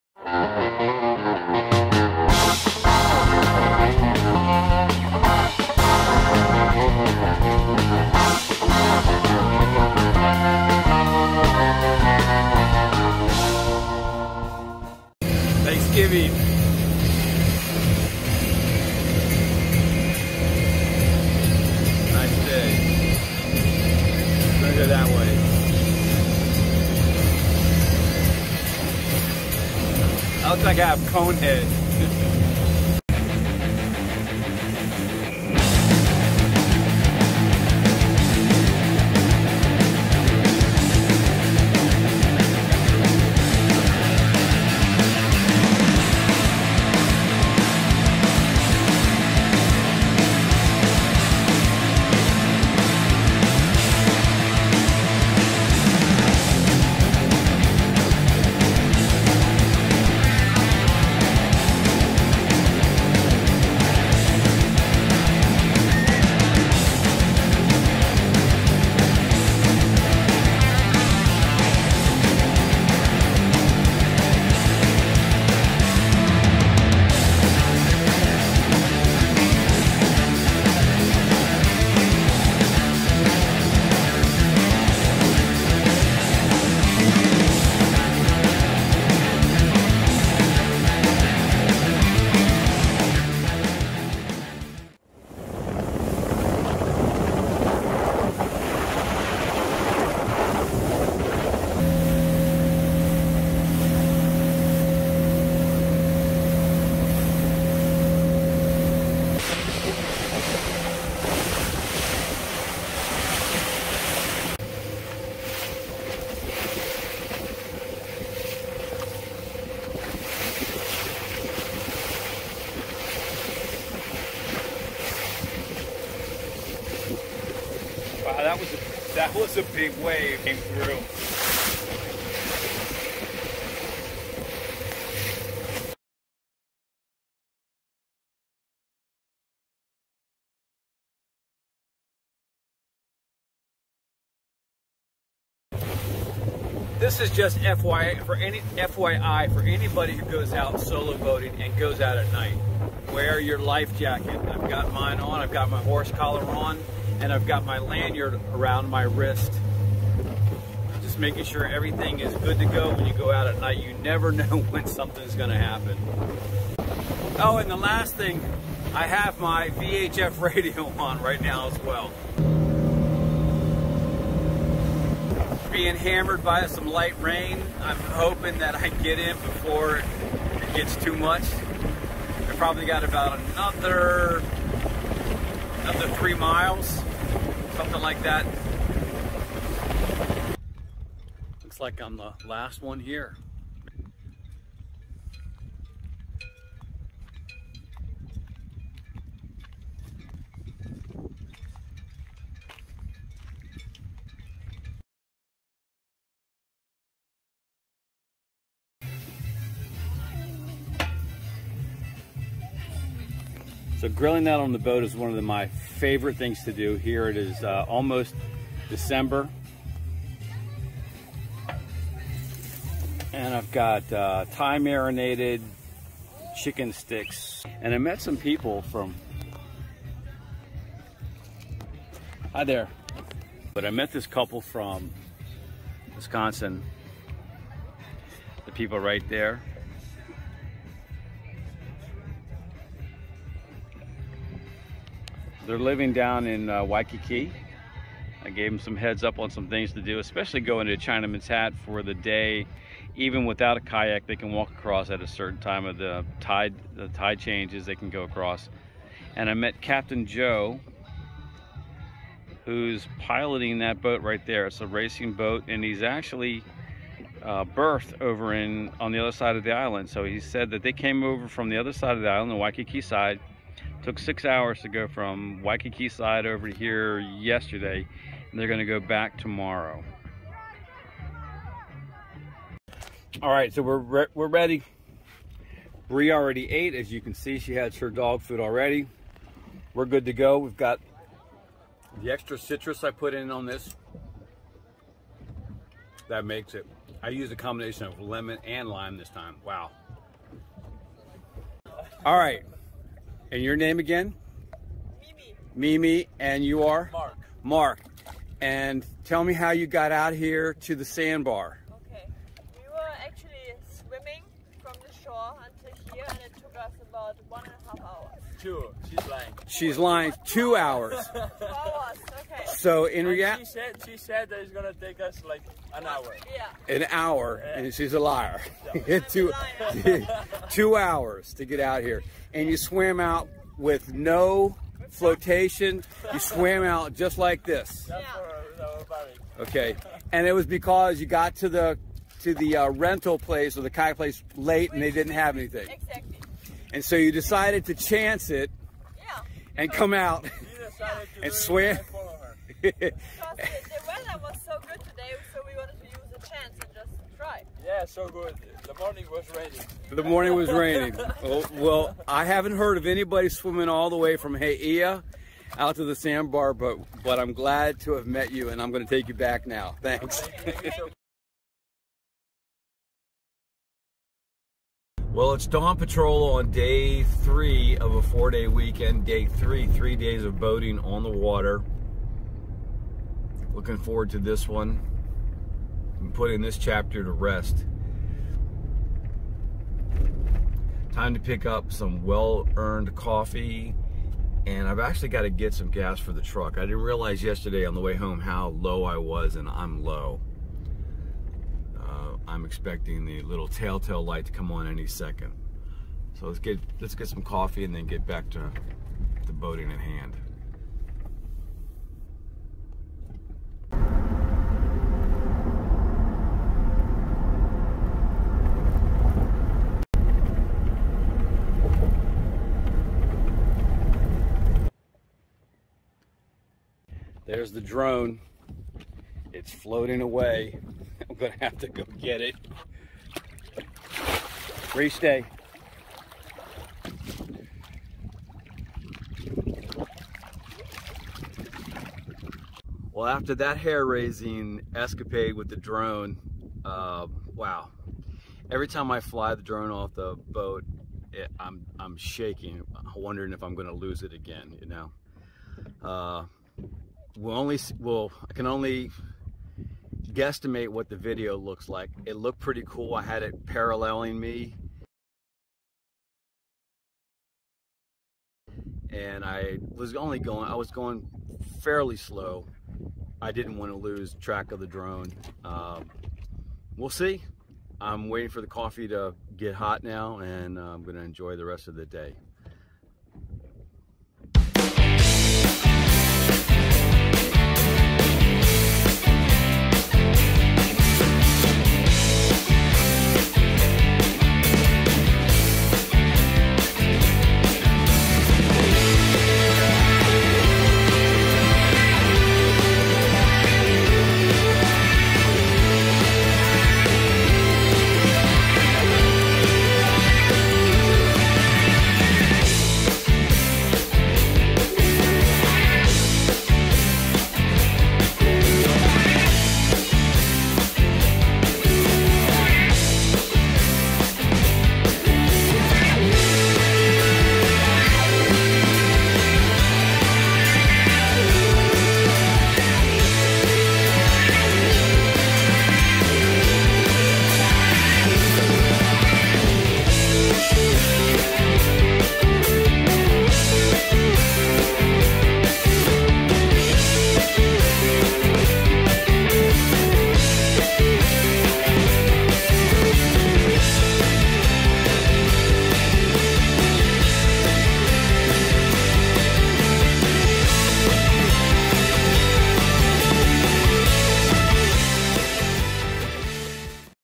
Best Boating Winter Holiday Weekend With A Yamaha Jet Boat
Can't go home for the holidays? No Worries Hit the water.  A 4 day weekend - 1 day to work on the Tacoma and 3 days on the water with the 2017 Yamaha SX190.  It didn't skip a beat.  Waves, Sight Seeing Up the Coast, Night Boating, Grilling, Meeting New People and Droning.

Features include Hawaii, Oahu, Yamaha Jet Boat, SX190.